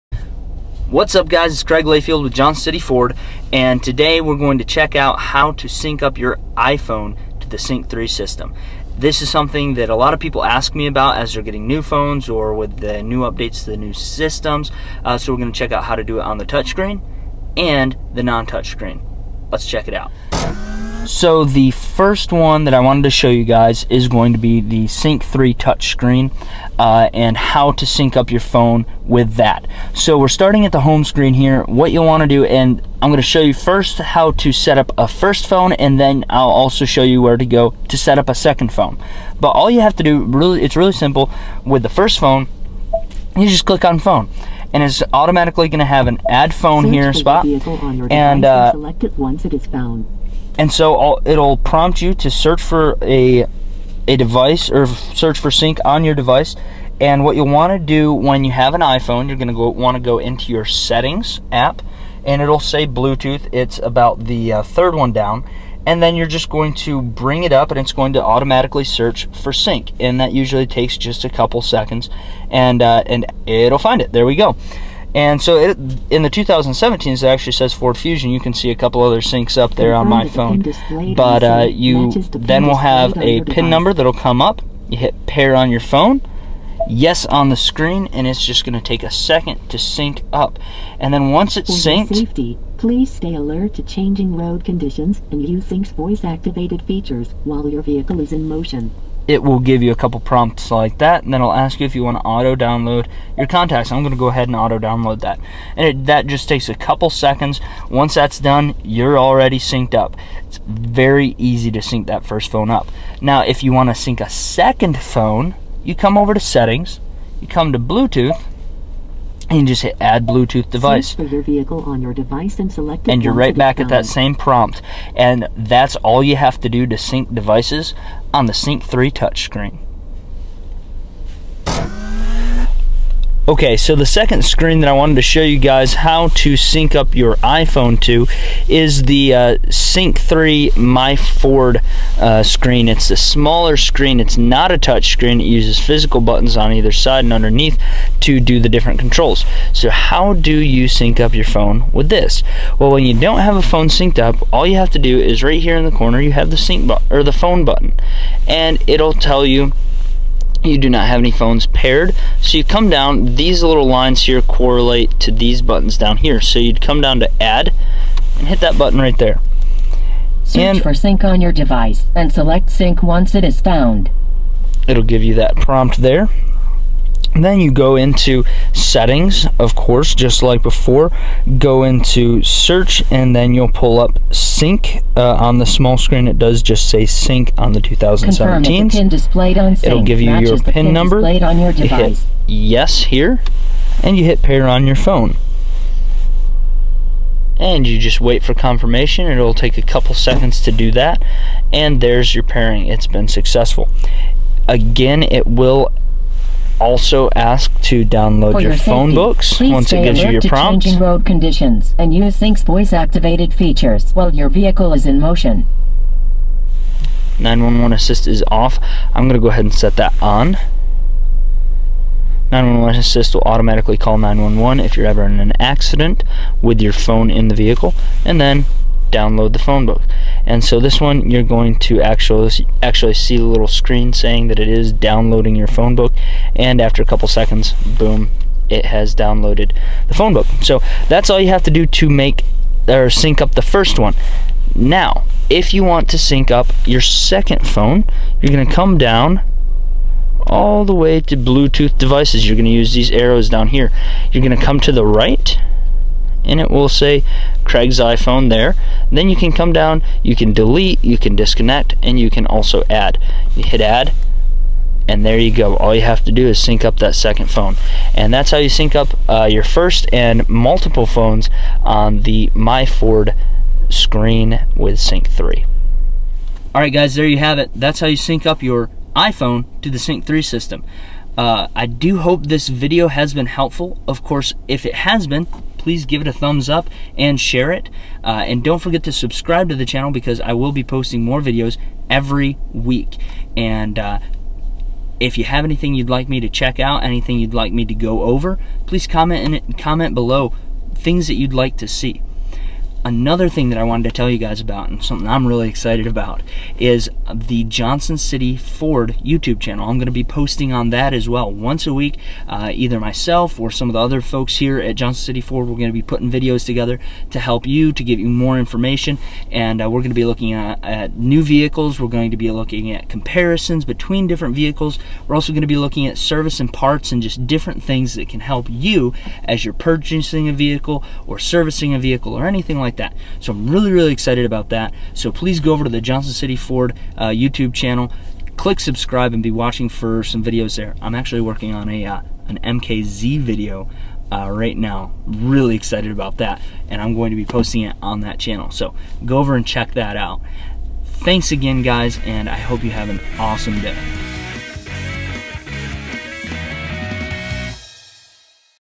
Quick Tips: How to Sync your iPhone to Sync 3 | Craig Layfield
Today we are going to check out how to sync your iPhone to the Sync 3 system- both with the Sync 3 touch screen and non-touch screen. Hope this helps!